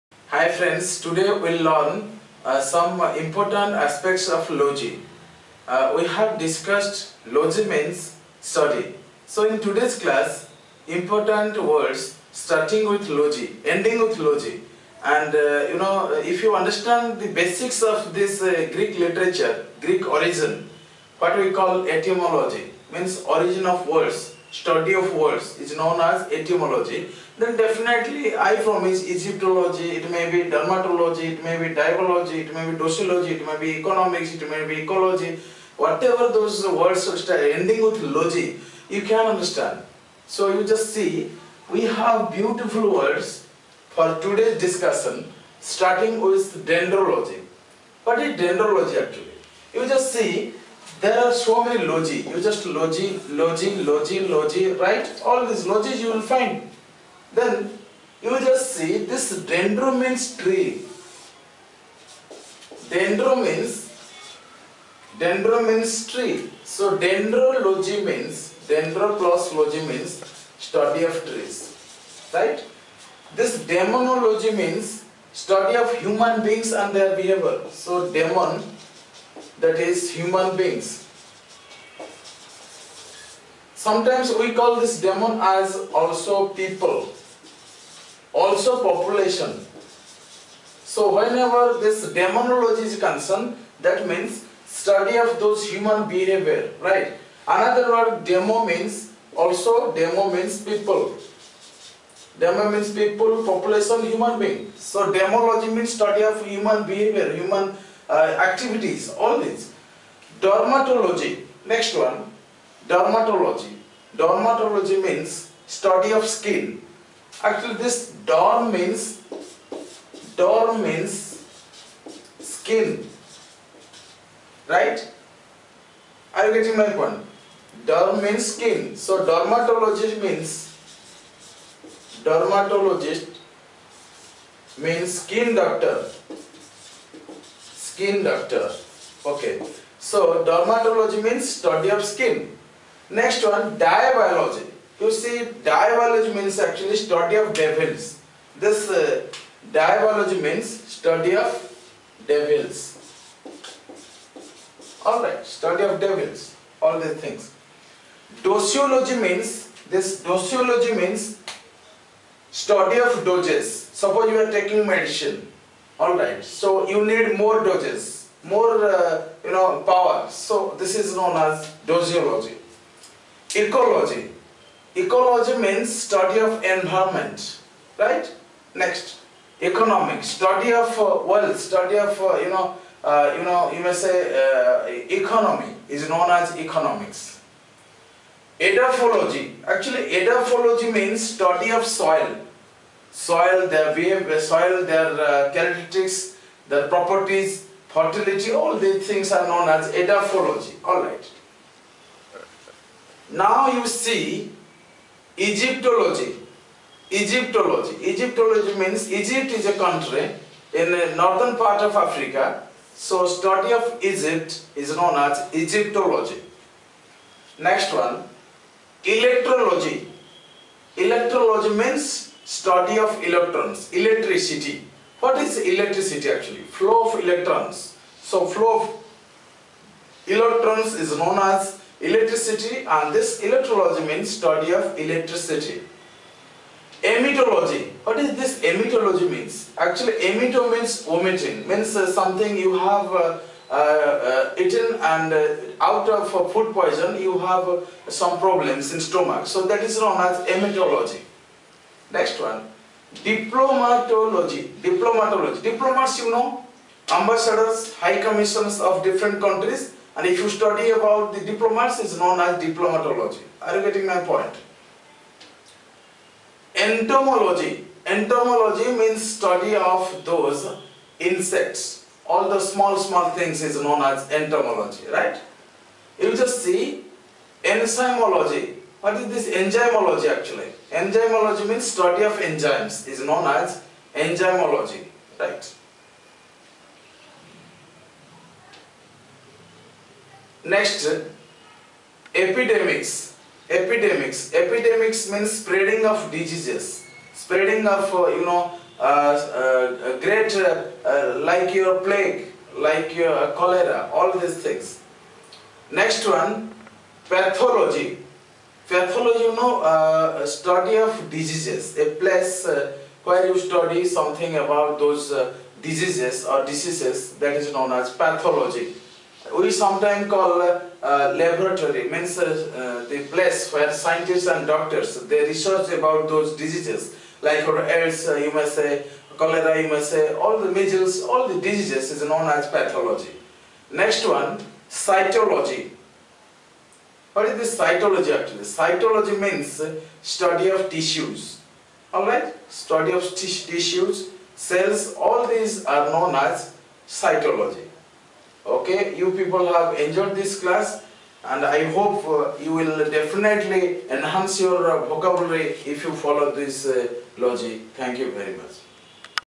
IMPROVE YOUR WORD POWER
LOGY is a Greek word means study...lets learn something about this for enhancing vocabulary.